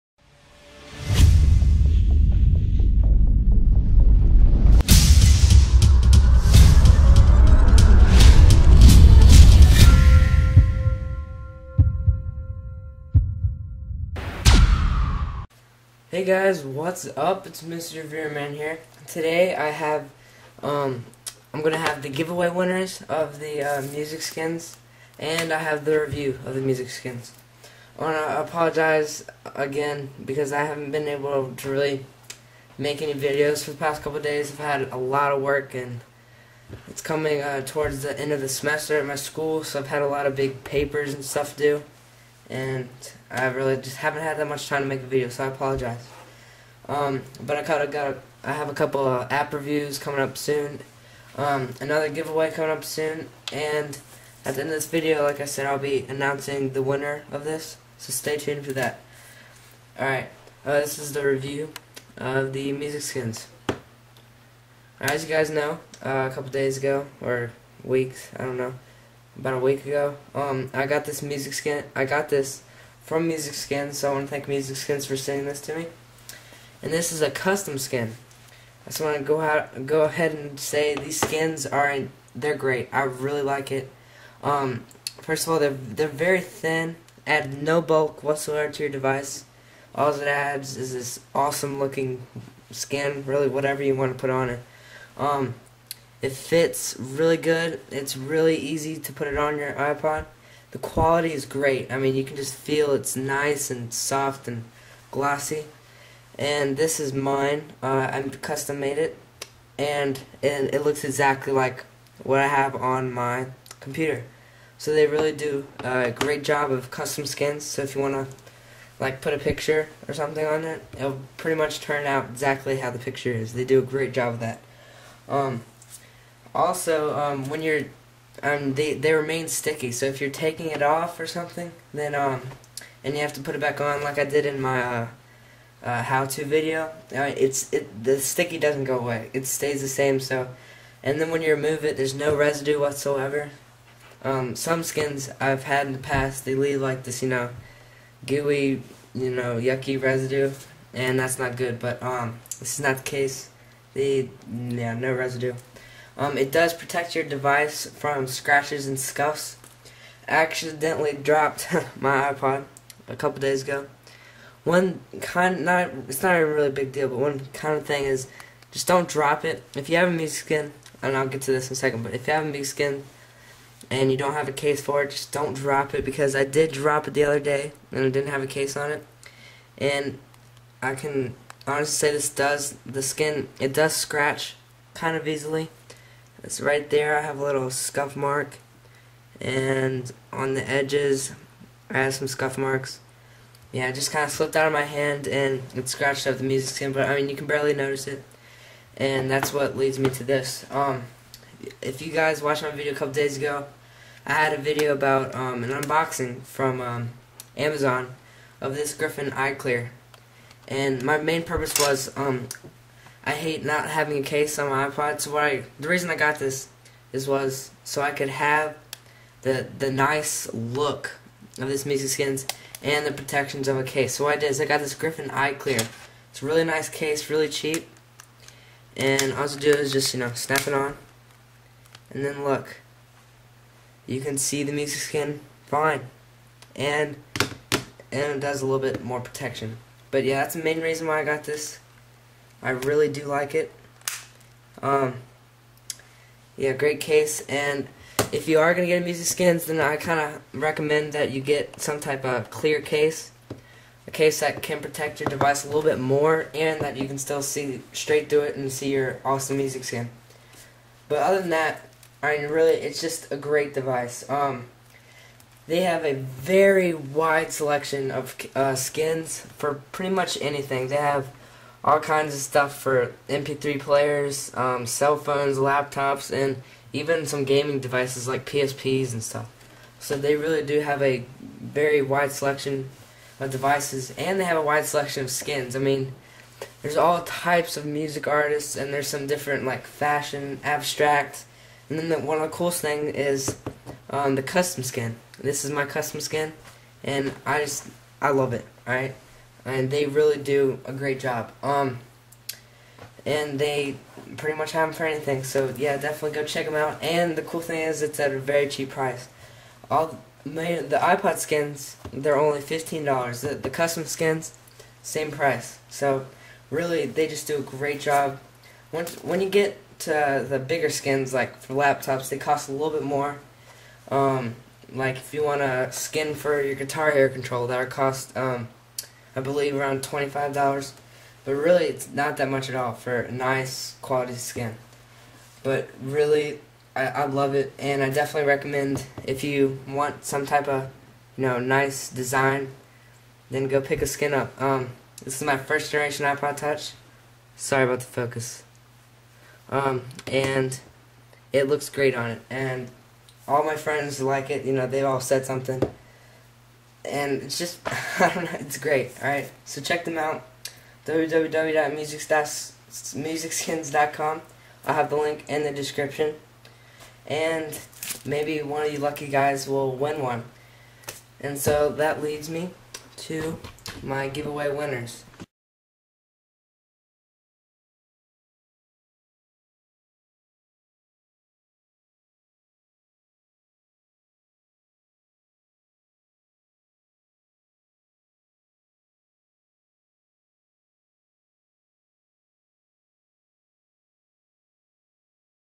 Review: Music Skins
This is a long awaited review of Music Skins, i haven't had time to make this video, i have been extremely busy with school!!